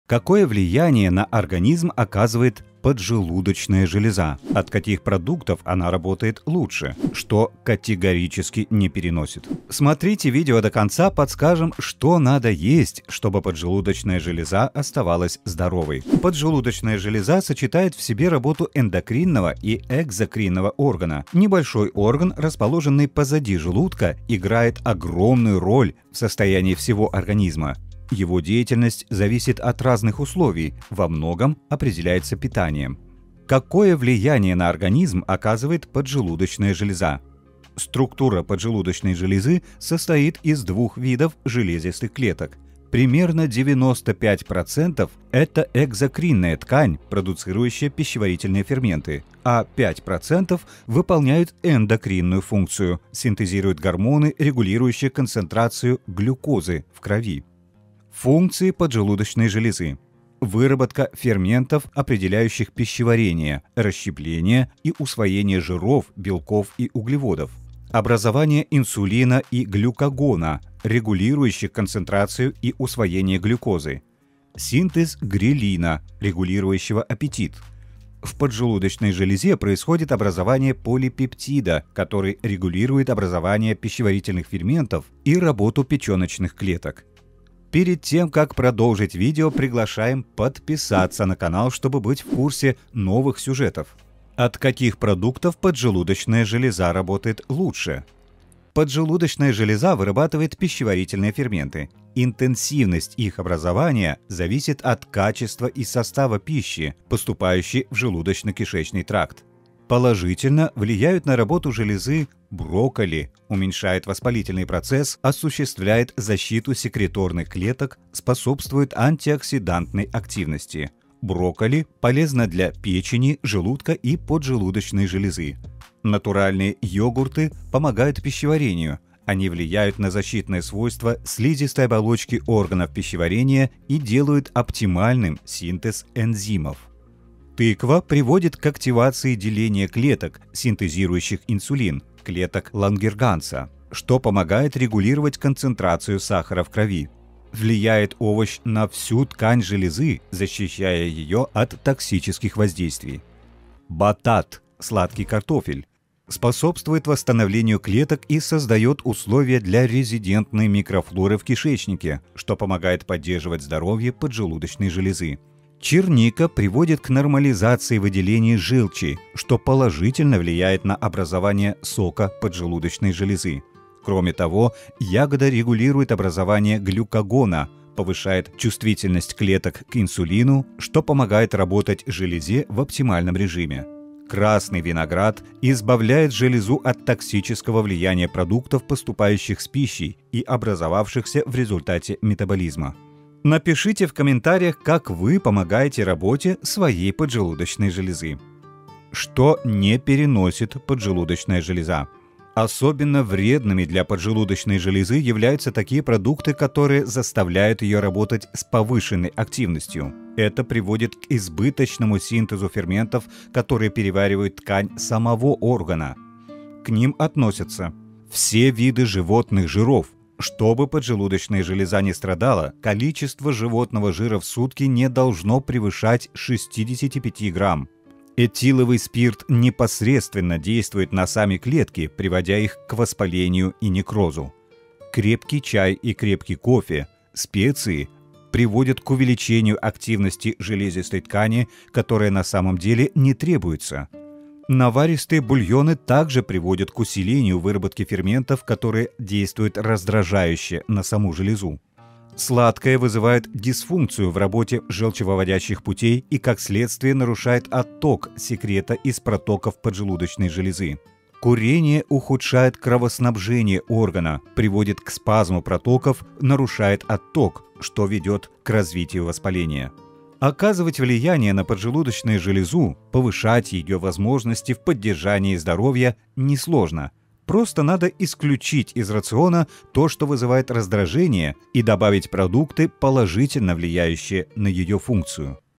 Врачи постоянно так делают! Для крепкого здоровья поджелудочной железы нужно добавить...
Какие продукты любит поджелудочная железа. Какое влияние на организм оказывает поджелудочная железа? От каких продуктов она работает лучше? Что категорически не переносит? Смотрите видео до конца, подскажем, что надо есть, чтобы поджелудочная железа оставалась здоровой.

Что не переносит поджелудочная железа?
Особенно вредными для поджелудочной железы являются такие продукты, которые заставляют её работать с повышенной активностью. Это приводит к избыточному синтезу ферментов, которые переваривают ткань самого органа.
К ним относятся:
• Все виды животных жиров, чтобы поджелудочная железа не страдала, количество животного жира в сутки не должно превышать 65 г;
• Этиловый спирт непосредственно действует на сами клетки, приводя к их воспалению и некрозу;
• Крепкий чай и крепкий кофе, специи приводят к увеличению активности железистой ткани, которое на самом не требуется;
• Наваристые бульоны также приводят к усилению выработки ферментов, которые действуют раздражающе на саму железу;
• Сладкое вызывает дисфункцию в работе желчевыводящих путей и как следствие нарушает отток секрета из протоков поджелудочной железы;
• Курение ухудшает кровоснабжение органа, приводит к спазму протоков, нарушает отток, что ведет к развитию воспаления.
Оказывать влияние на поджелудочную железу, повышать её возможности в поддержании здоровья не сложно!
Просто надо исключить из рациона то, что вызывает раздражение и добавить продукты, положительно влияющие на её функцию!

➤➤➤О НАС:
Канал посвящен здоровью:

➤➤➤ВНИМАНИЕ!